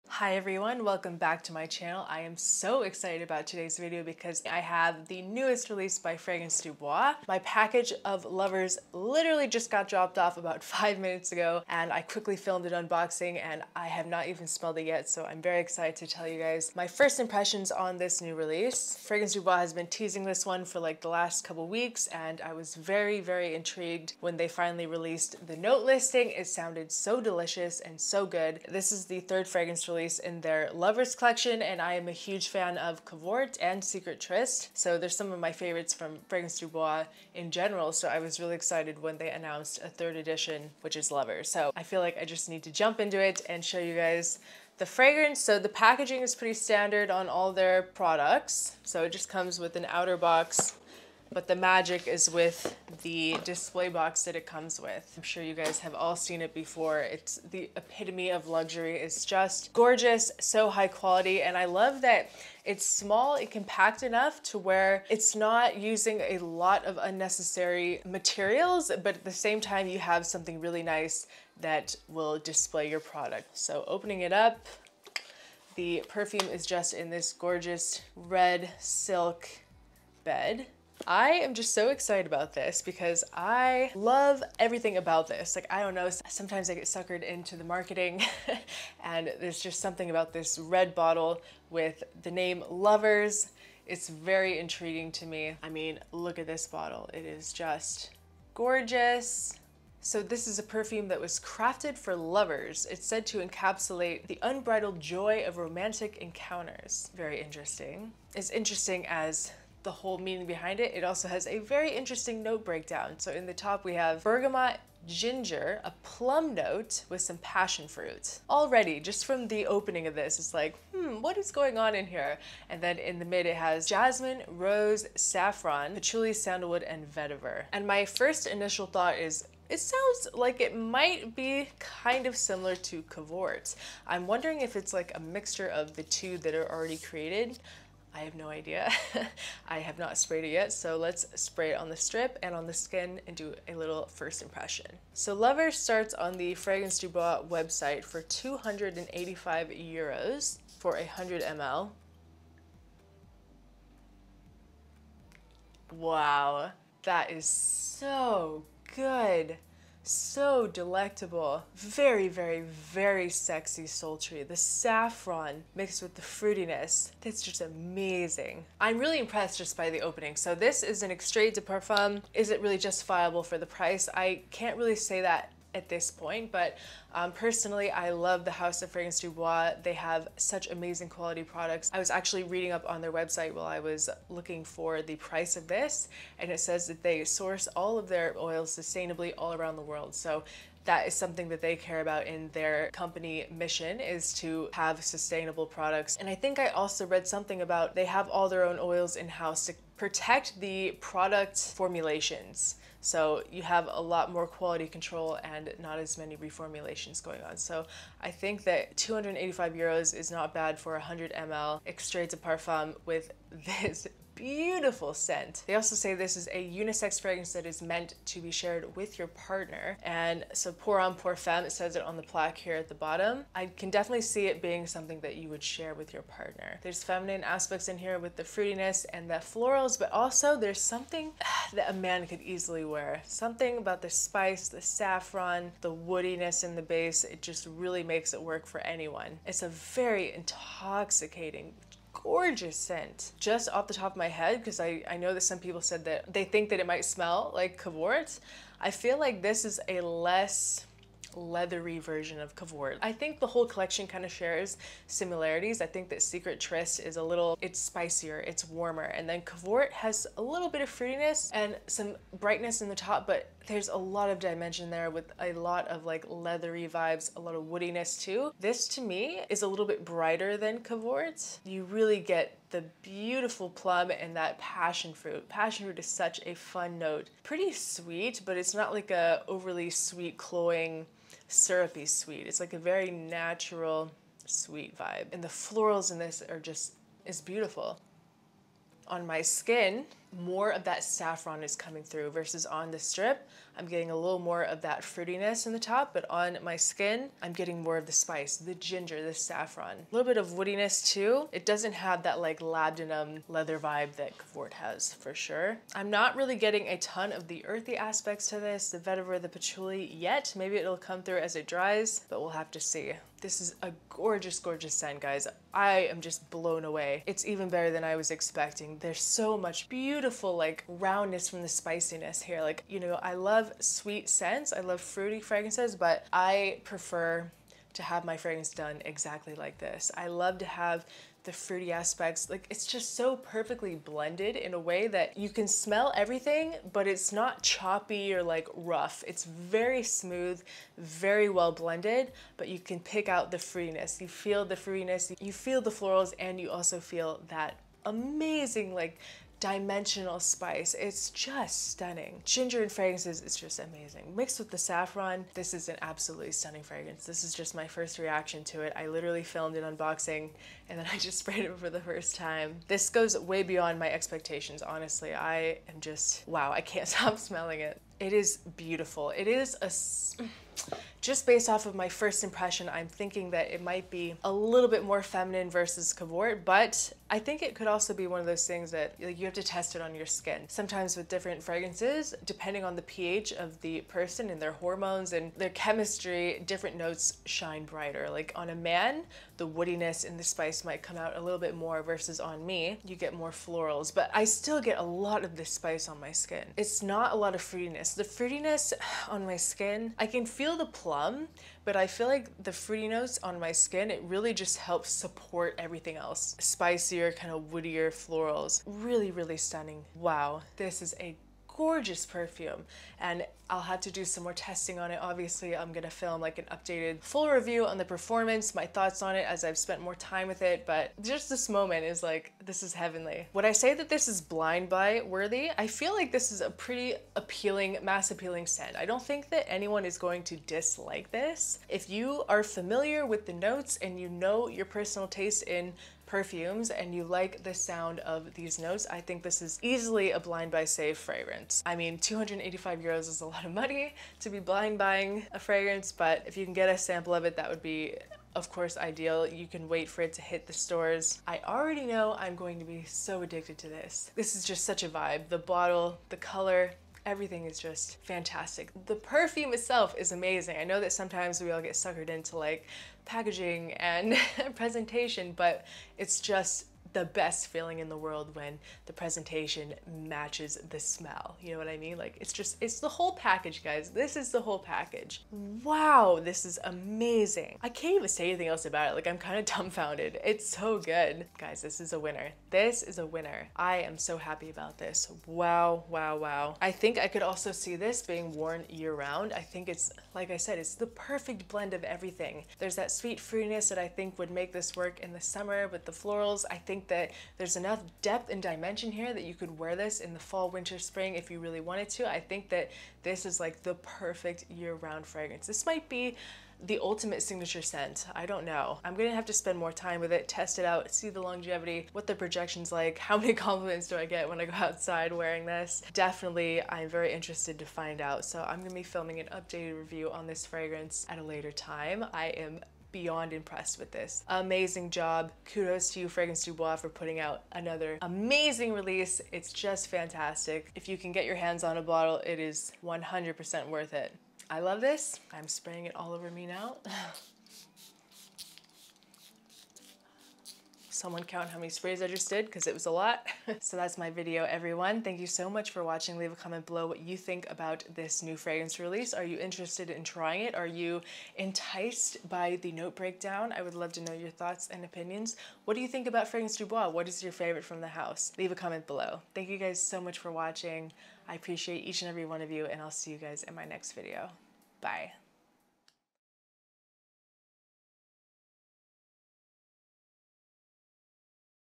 *NEW RELEASE* LOVERS by Fragrance du Bois My HONEST First Impressions! Niche Unisex Fragrance Review
My Jewelry Company: